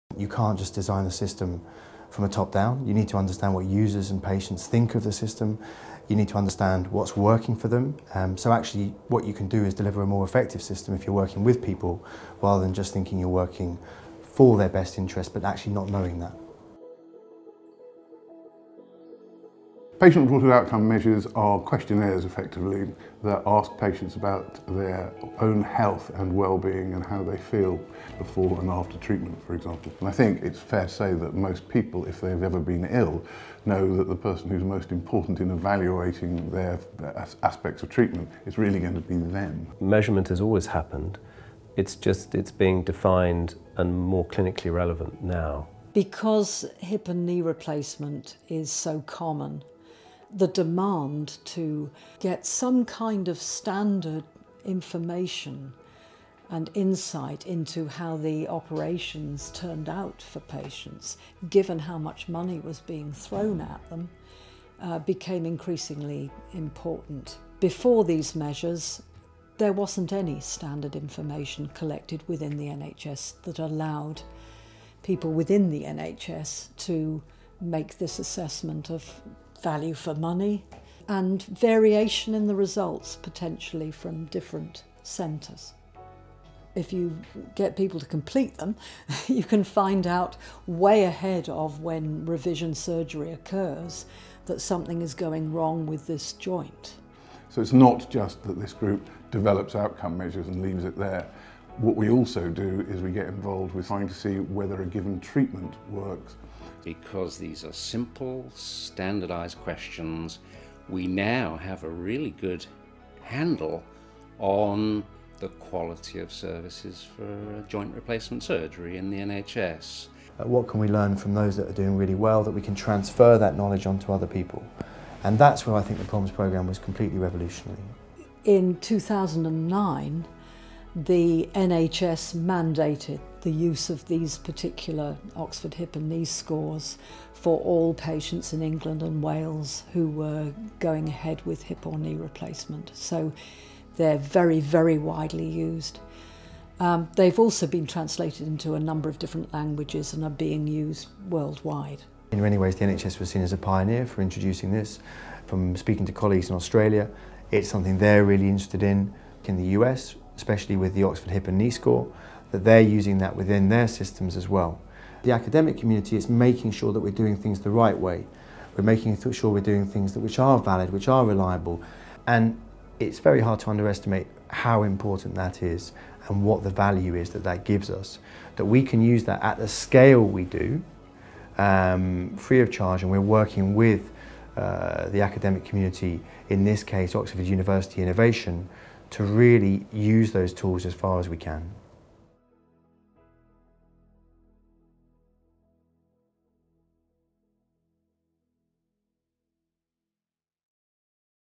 Keeping the Patient in the Picture: Patient Reported Outcome Measures
Patient choice over treatment and care is a central feature of the UK’s National Health System, and patients' experience of treatment and care is a major indicator of quality. But it isn’t easy to find out how well a treatment has worked.

Find out how a group of Oxford University researchers pioneered the use of questionnaires about hip and knee replacements, and revolutionized how patients’ experience could be used to assess and improve the UK’s healthcare.

The rights to the hip and knee questionnaires are owned by Oxford University Innovation: innovation.ox.ac.uk

More on how PROMs are used in the NHS